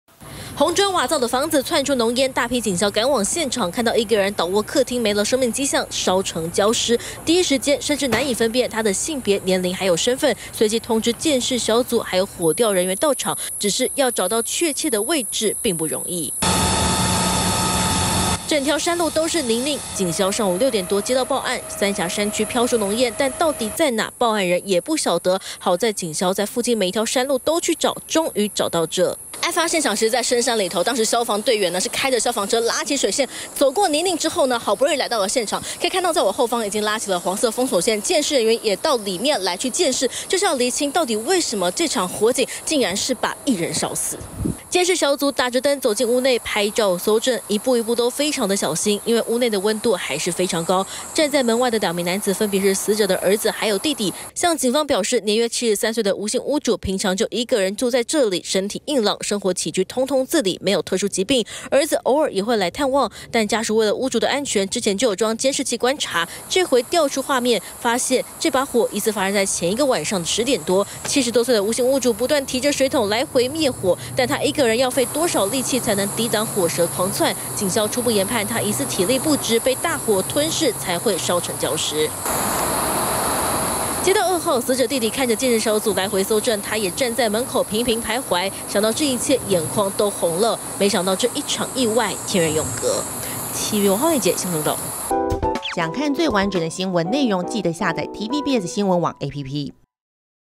疑返家滅火體力不支 翁倒臥屋內遭燒死｜TVBS新聞
新北市三峽發生一起火警，雖然警消到場，火勢已經熄滅，但清理火場時發現，屋主已經變成焦屍，初步了解，平常死者獨居，疑似火災發生後，可能逃生後又回去滅火，才會倒在門旁，葬身火窟。